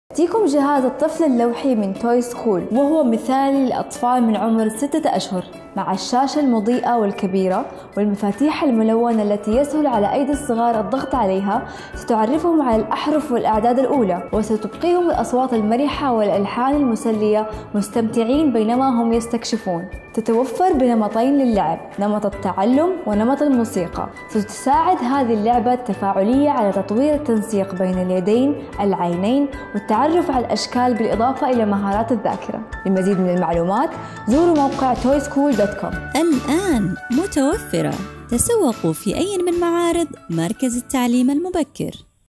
لوح تعليم الطفل
إن لوح تعليم الطفل من توي سكول هي لعبة تفاعلية لطيفة سوف يحبها طفلك. مع وضع الإختيار بين التعلم والموسيقى سيعرف لوح التعليم طفلك على الحروف الأبجدية و الأرقام وحتى الأشكال.  وتكافئه الألحان  المرحه والاصوات الممتعه عند الضغط على الزر، وتعليمهم المهارات الحركية الدقيقة أثناء اللعب. هذه اللعبة مناسبة للأطفال من عمر 6 أشهر.